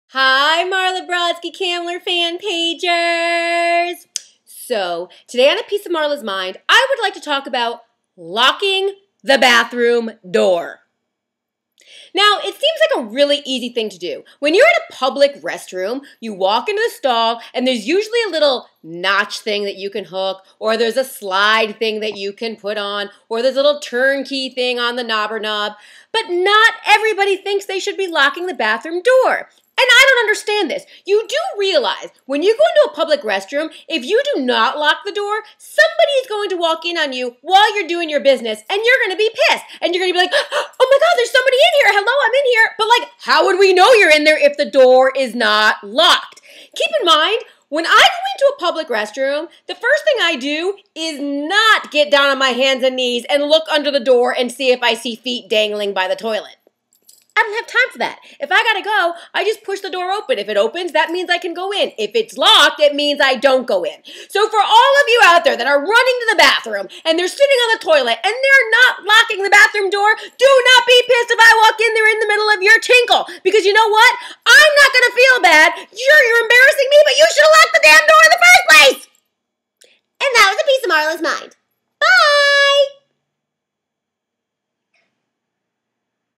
A PIECE OF MARLA'S MIND: INSTALLMENT 60
Somewhat EXCLUSIVELY for the Marla Brodsky Kamler fan pagers on facebook! Today Marla shares her views on LOCKING THE BATHROOM DOOR!